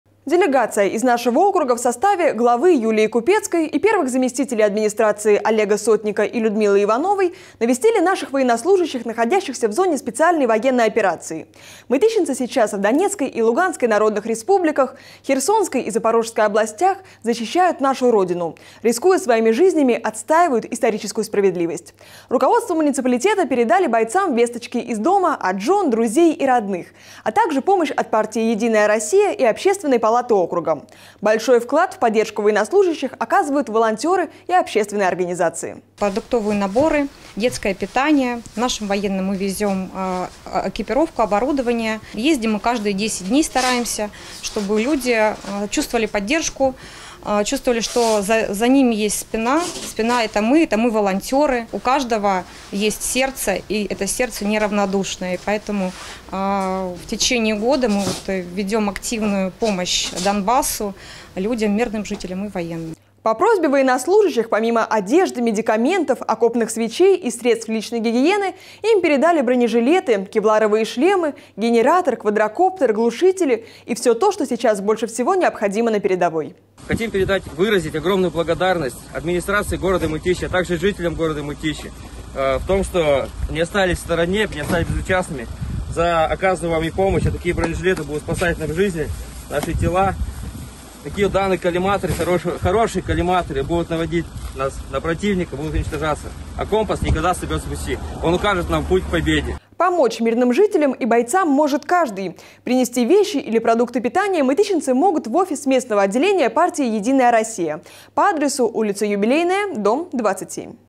Делегация из Мытищ навестила военнослужащих, находящихся в зоне СВО
Делегация из нашего округа в составе главы Юлии Купецкой и первых заместителей администрации Олега Сотника и Людмилы Ивановой навестили наших военнослужащих, находящихся в зоне специальной военной операции. Мытищинцы сейчас в Донецкой и Луганской Народных республиках, Херсонской и Запорожской областях защищают нашу Родину, рискуя своими жизнями, отстаивают историческую справедливость. Руководство муниципалитета передали бойцам весточки из дома от жен, друзей и родных, а также помощь от партии «Единая Россия» и Общественной палаты округа. Большой вклад в поддержку военнослужащих оказывают волонтёры и общественные организации. (12+)